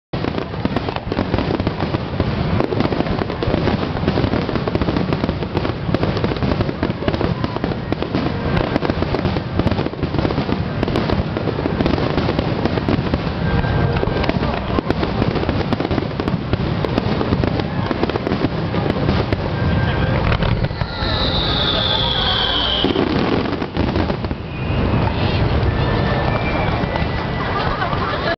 fireworks in Tokyo part4 江戸川花火大会
街中から撮影、観客の一人が戦争時の空襲はこんなものだったのかもと言い出すほど空は赤く染まり、爆弾のように見えた。